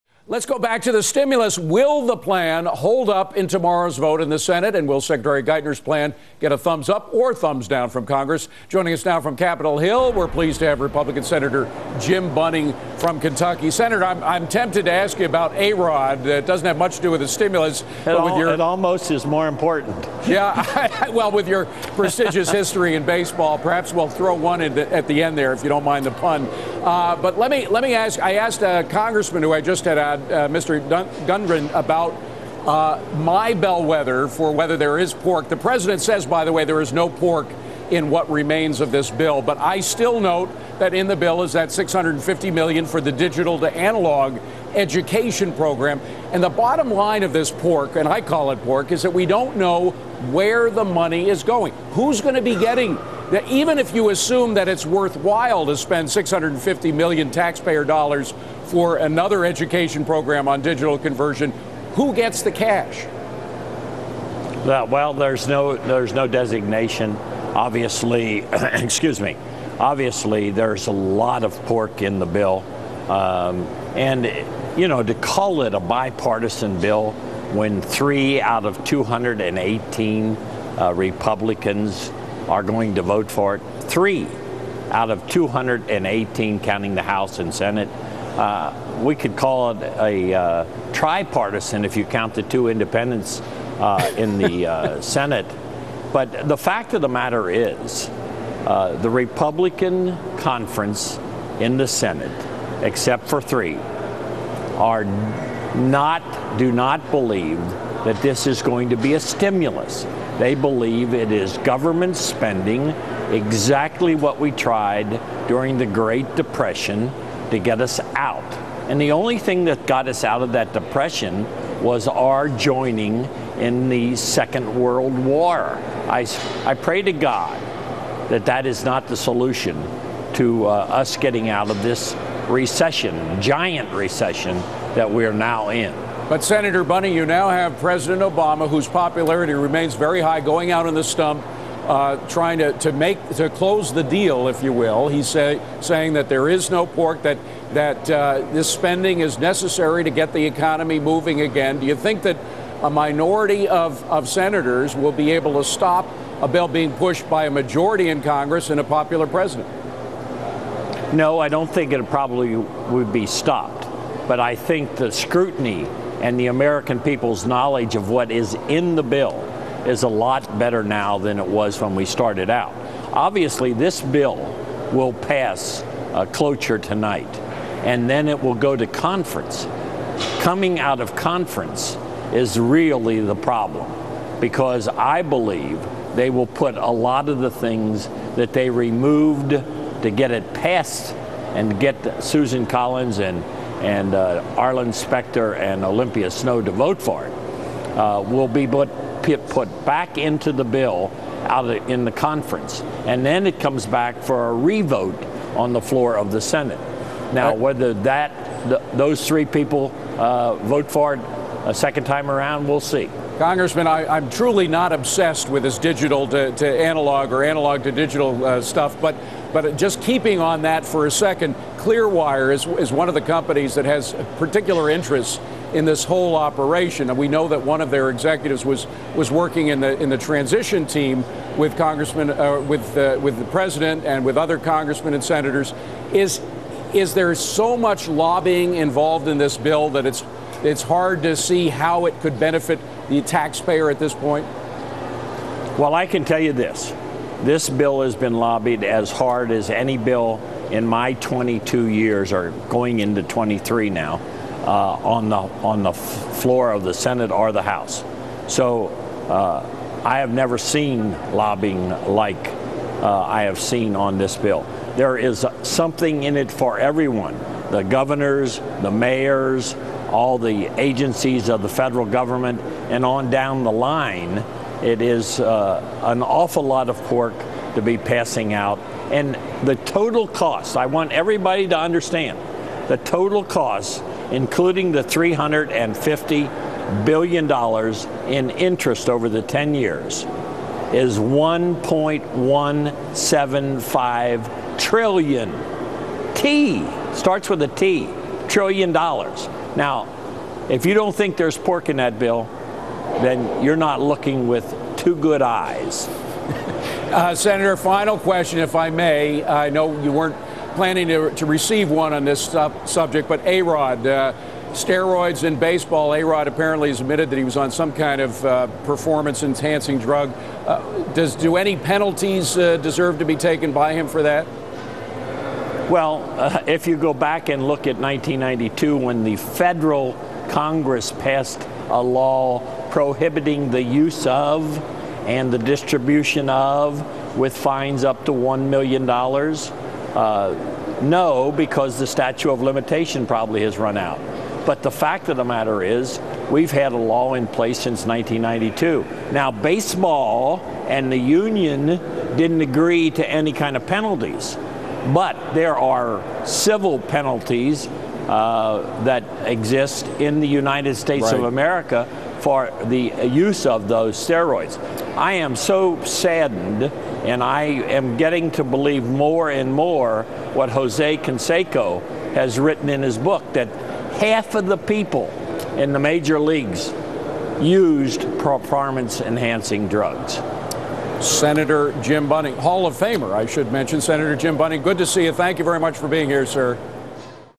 Senator Jim Bunning discusses wasteful spending in stimulus bill
U.S. Senator Jim Bunning discusses the wasteful spending in the stimulus bill and the fact that the bill is not a bipartisan package that will stimulate the economy on the Fox Business Network.